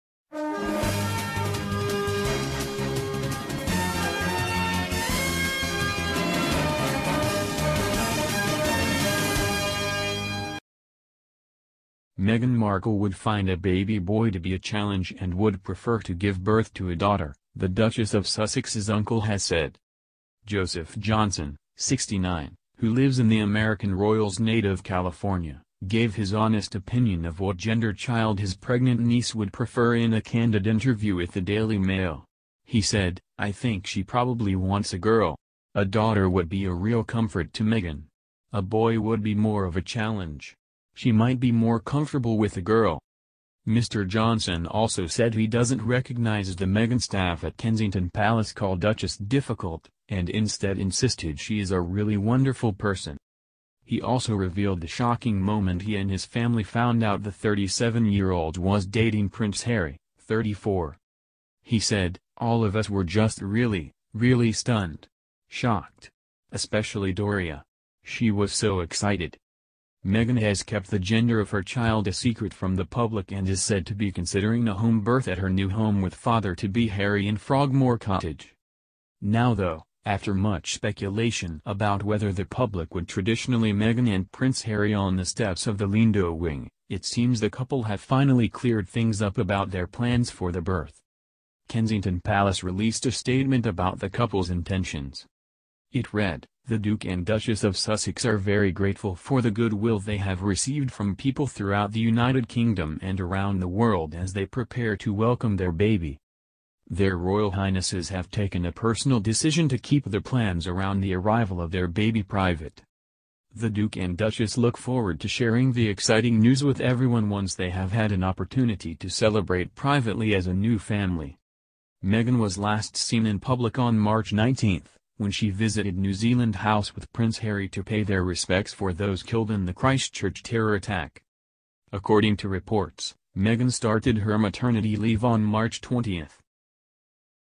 Shock royal claim: Meghan Markle faces the criticism of the queen if she gives birth to a baby girl
MEGHAN Markle and Prince Harry were banished to Frogmore Cottage because “the Queen want them out of Kensington Palace”, it has been claimed.